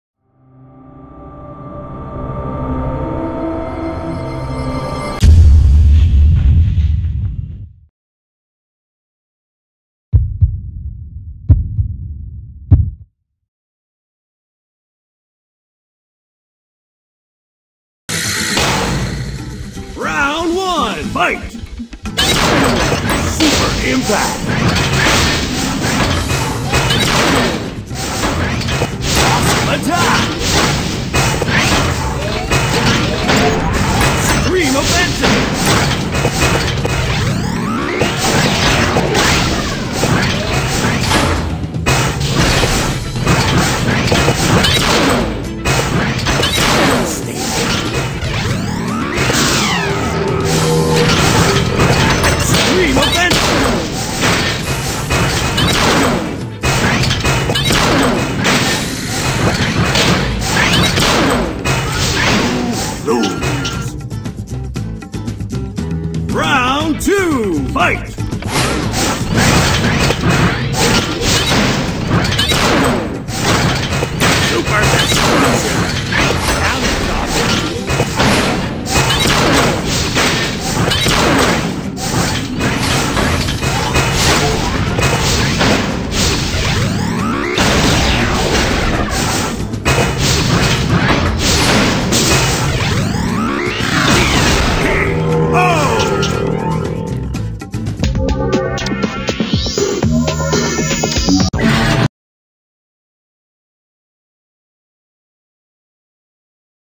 TKO, VILGAX VS SWAMPFIRE
Hey! here's my new series TKO , Titanic Kungfubot Offensive! , in this match I play as Vilgax versus MEFX who was playing Swampfire, okay! let the match begin!